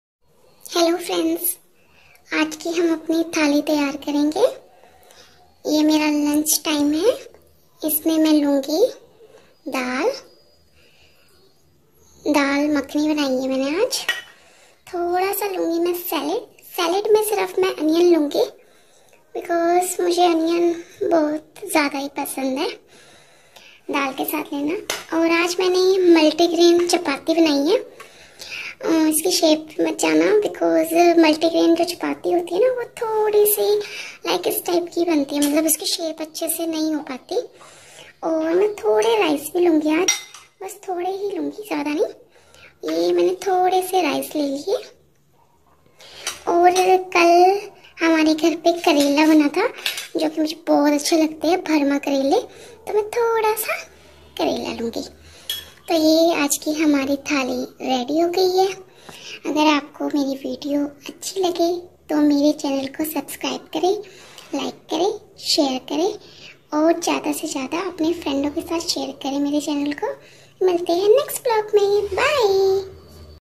5th day lunch tali ideas || 30 days lunch tali ideas || veg lunchbox
hii friend's aaj k lunch m apke liye daal makhni n roti or rice h agar apko meri ye lunch tali achi lgi toh mere channel ko subscribers kre or haa video ko subscribers kre or like n share jurr he kre .